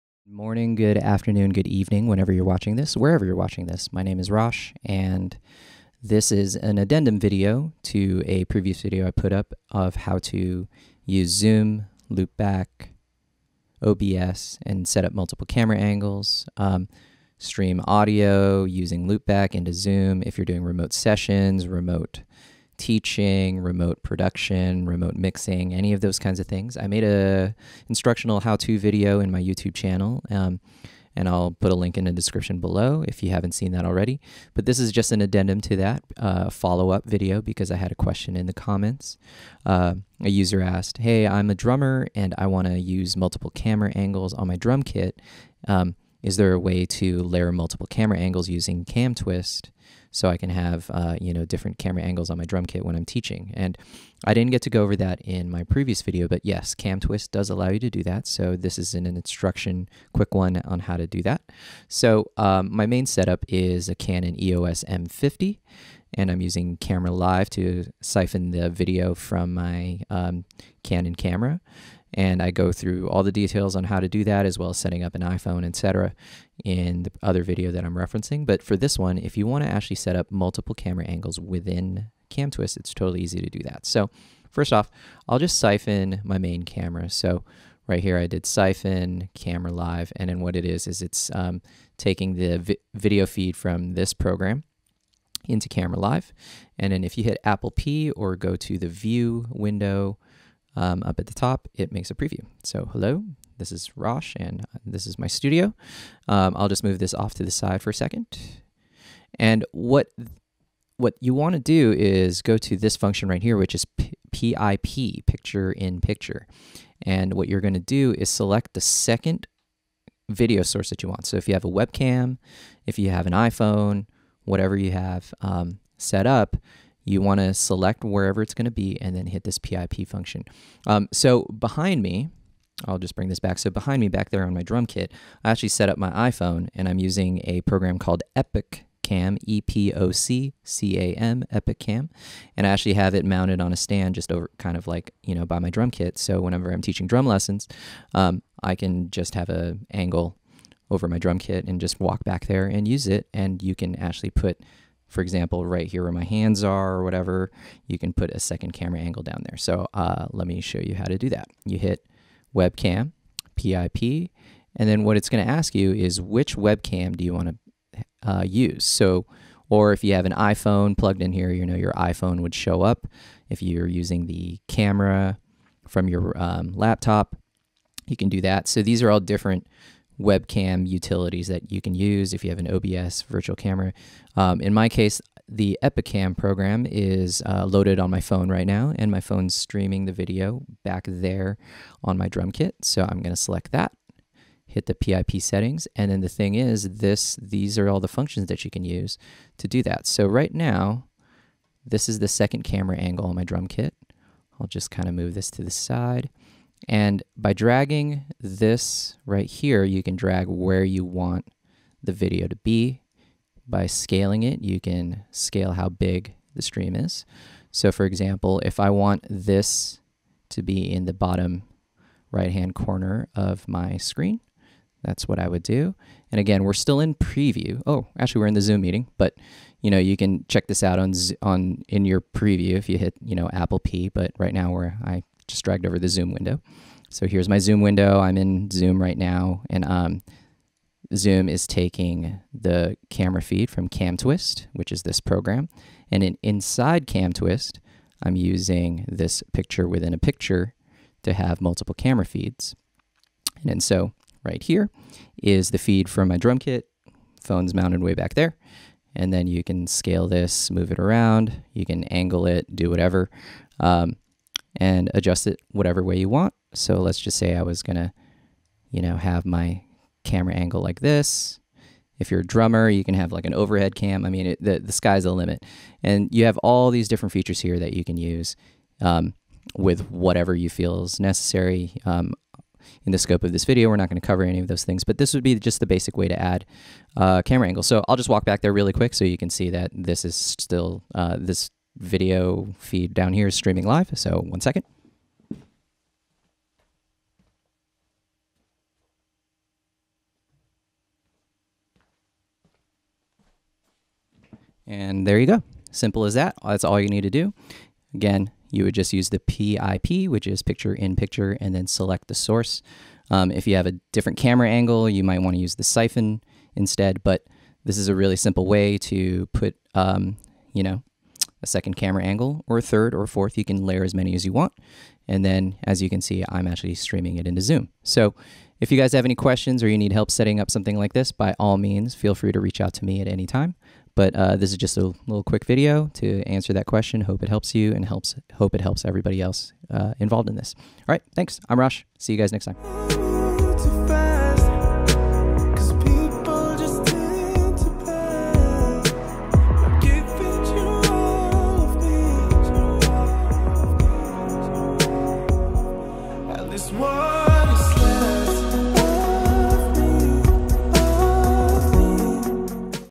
Using Multiple Cameras with Camtwist for Zoom.
This tutorial is designed to show how to use the "picture in picture" function on Camtwist to nest multiple video sources for livestreams, remote teaching, sessions etc on zoom.